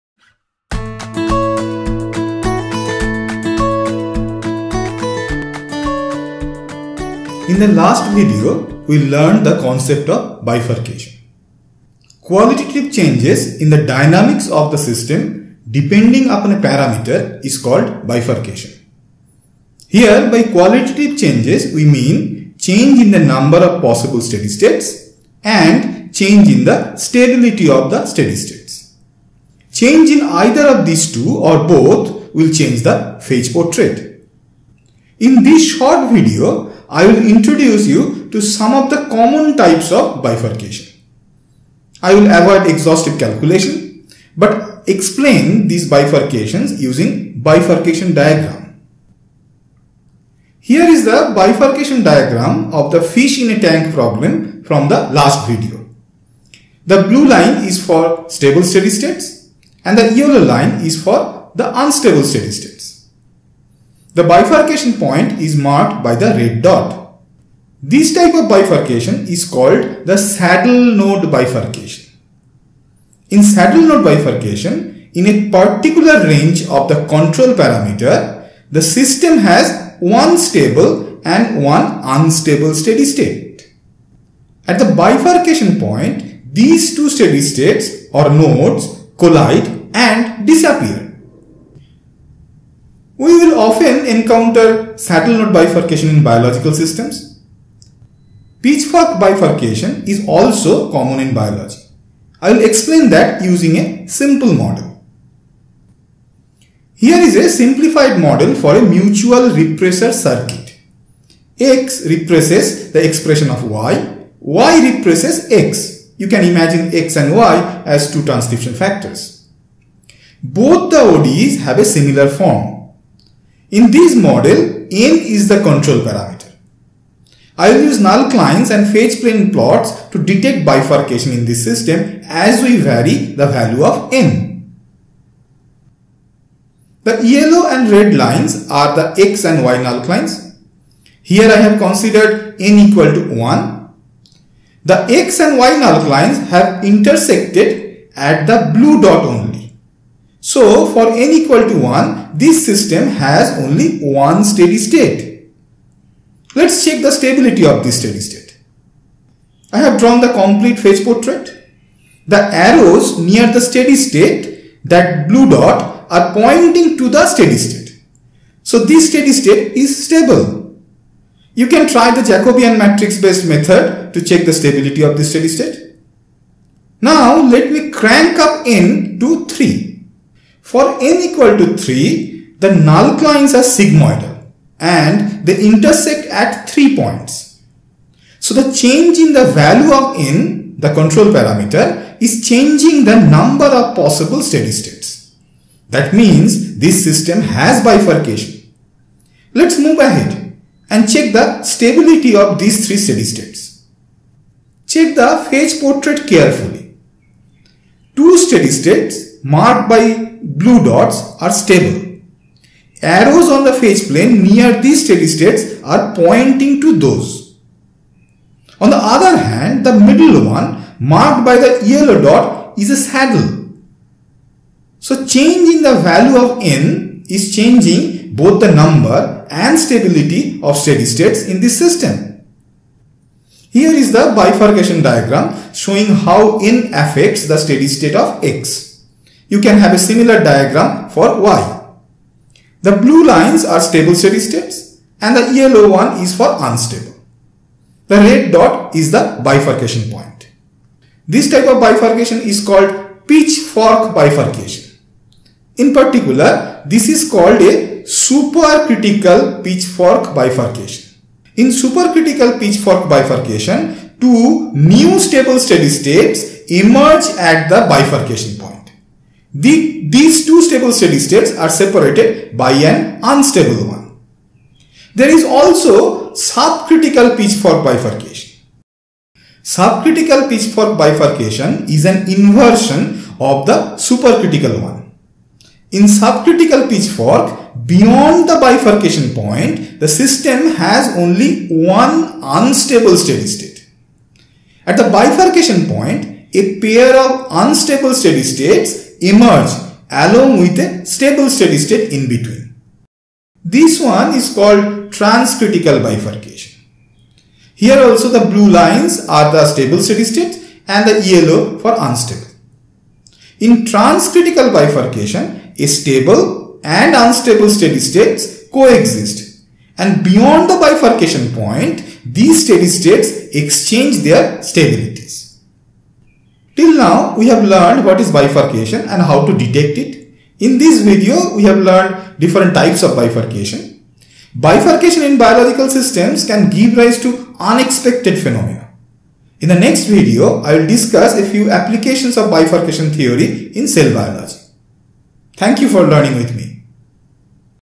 Systems Biology 5.2: Different Types of Bifurcation
This video is part of my lecture series on Systems Biology.

0:00 - Intro
0:51 - Saddle Node Bifurcation